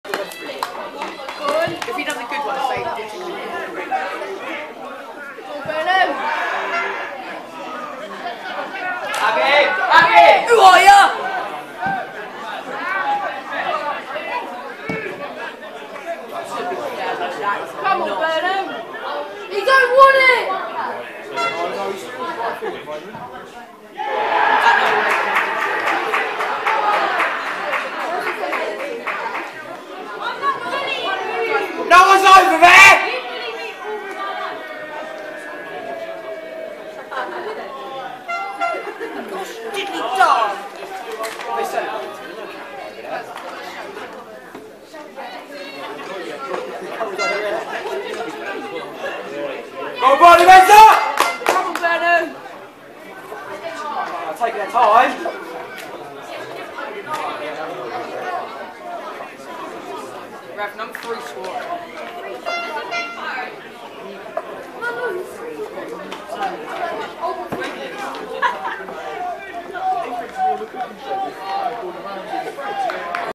Burnham Ramblers 2-3 Stanway Pegasus. Thurlow Nunn Eastern Counties League South. Tue22Nov2022
A dramatic finish at Leslie Field to a fiery clash between two sides vying to go top, as Burnham come from behind to lead at the break, and look set to tough it out for the win, until two injury times goals see Stanway snatch victory to hit the summit again.